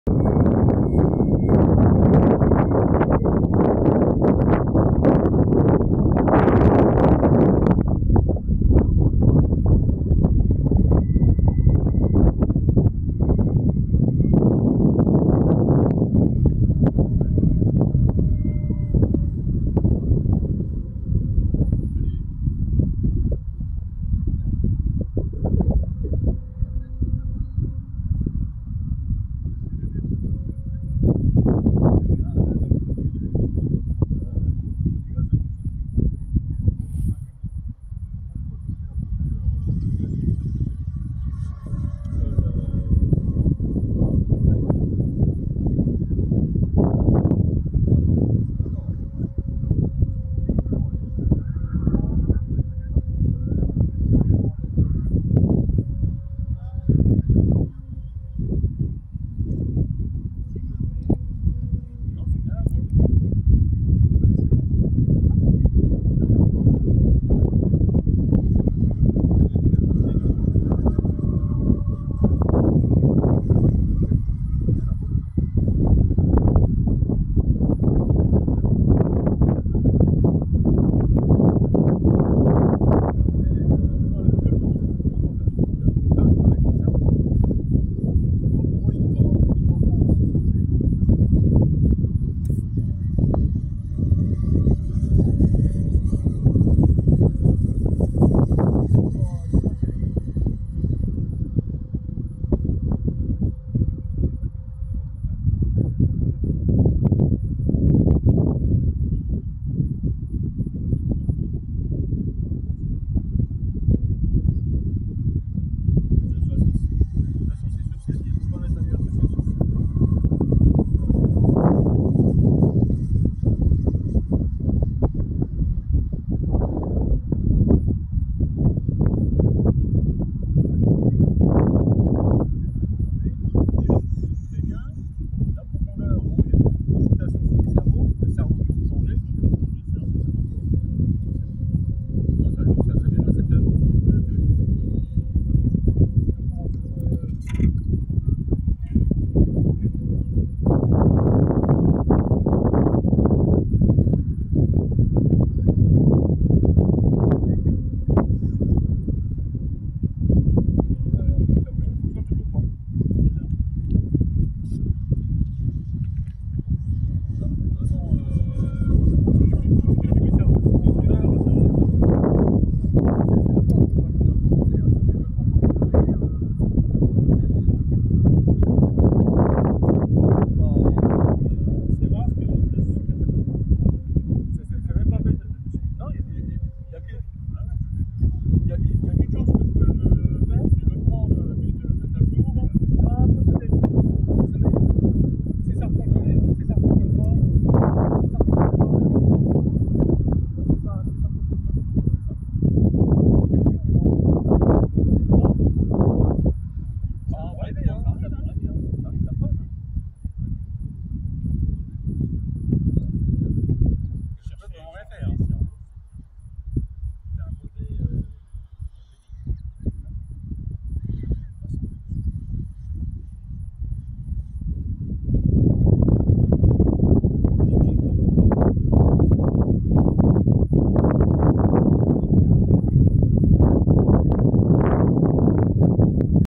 Vol avion type Marchetti
Premier vol de la semi maquette de l'avion d'entrainement type Marchetti